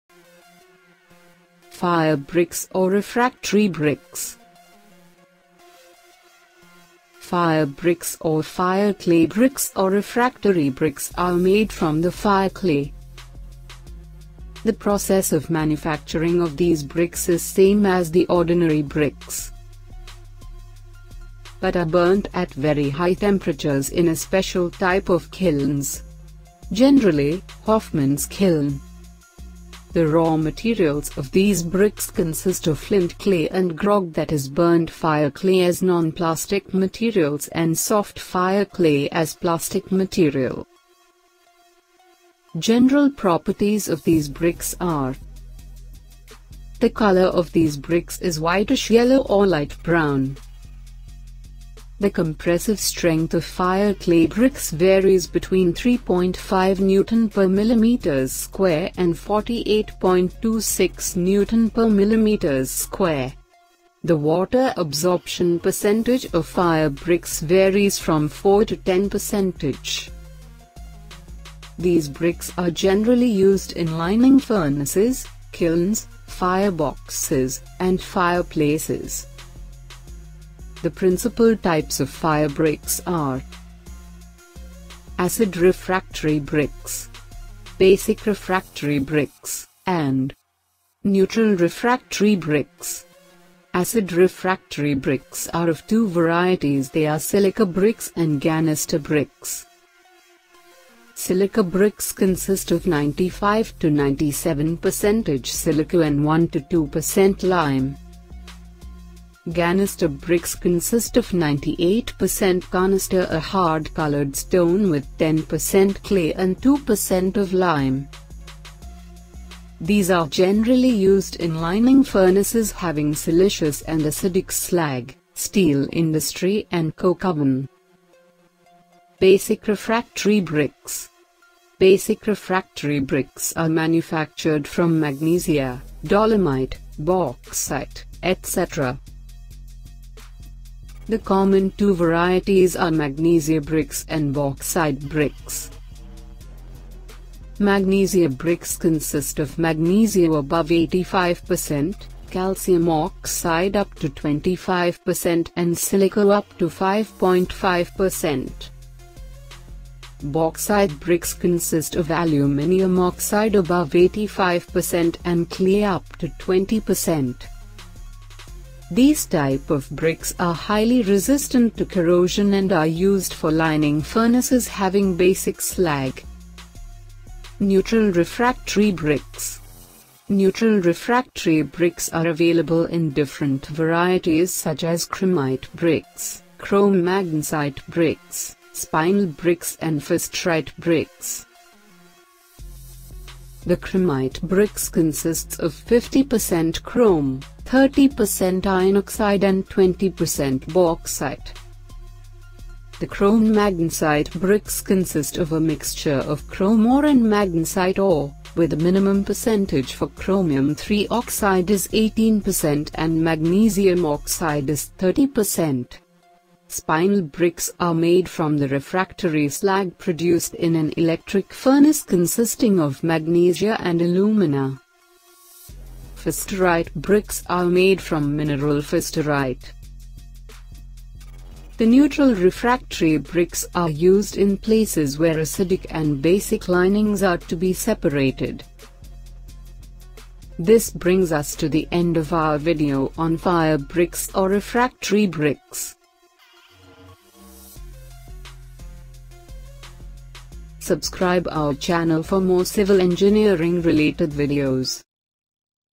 Fire Bricks or Fire-Clay bricks or Refractory bricks // Types of Fire Bricks or Refractory Bricks //
🔥 In this informative video, we dive into the fascinating world of FireBricks, FireClayBricks, and RefractoryBricks. Join us as we explore the properties, manufacturing process, and various applications of these essential materials in construction and industrial settings. Discover how fire bricks withstand extreme temperatures, providing excellent insulation and protection against heat and fire hazards. Whether you're a DIY enthusiast or a professional builder, this video is a must-watch to enhance your knowledge about these resilient building components.